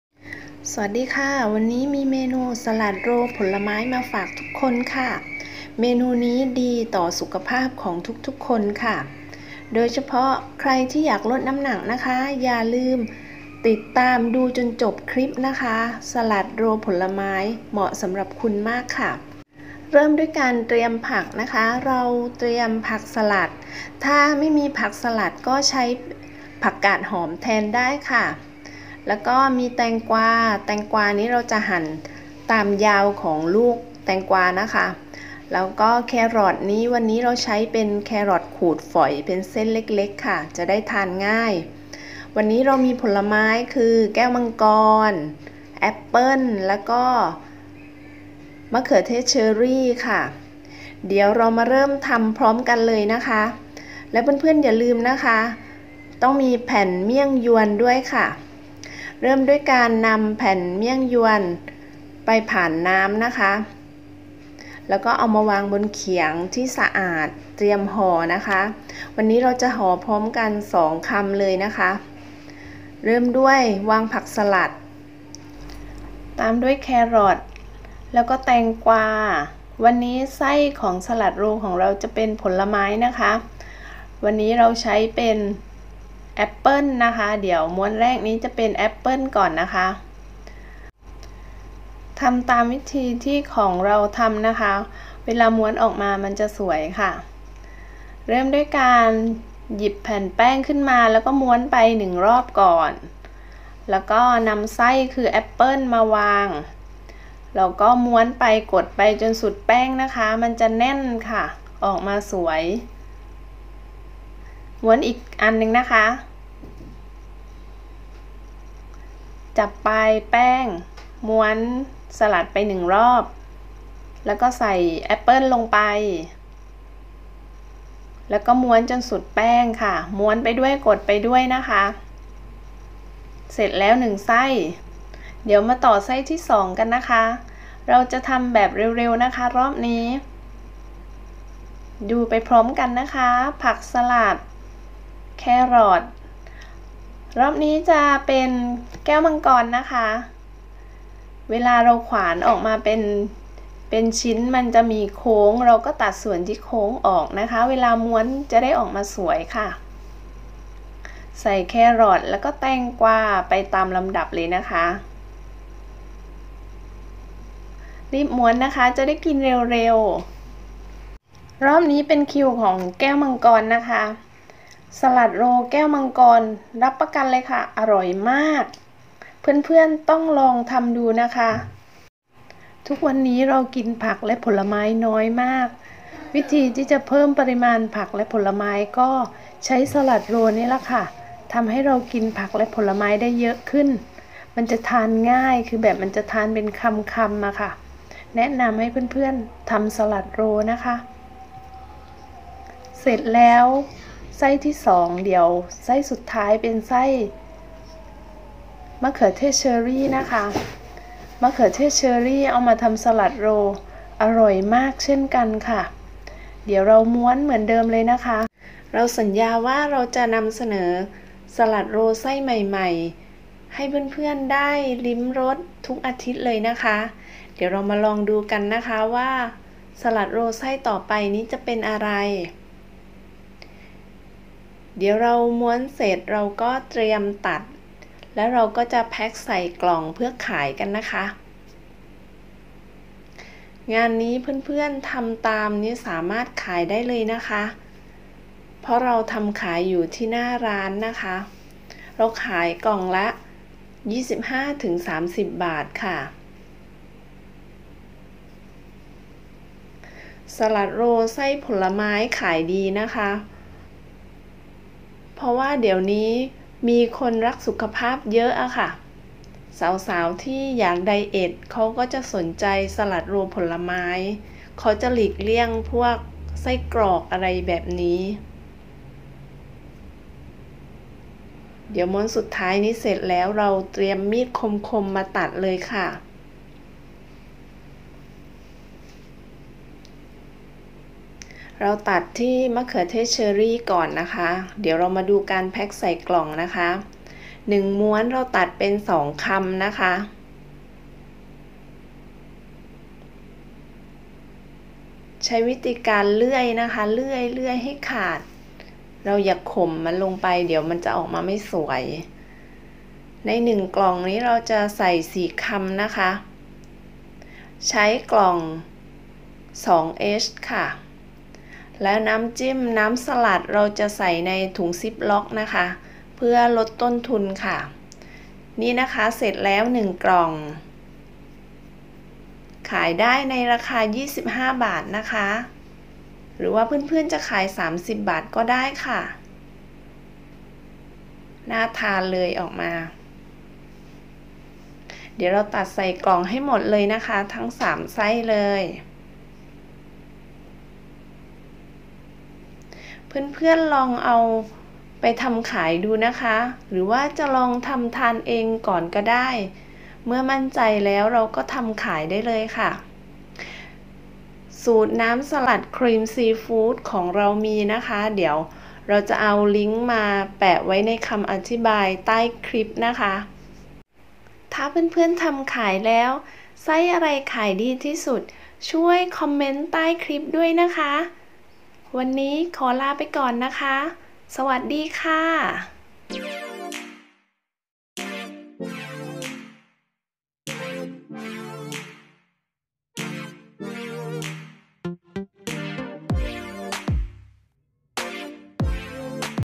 สลัดโรลผลไม้ รายได้เสริมเลิกงาน ทำอะไรขายดี EP16 สลัดโรล ผลไม้
เอาเมนูสลัดผลไม้มาฝากเพื่อนๆคะ...
ทำไปพร้อมกันเลยน่ะ

คลิปสอนทำ น้ำสลัดครีมซีฟู้ด

เพื่อนๆลองทำทานดูนะ รสชาติเป็นยังไง คอมเมนต์มาบ้างน่ะคะ...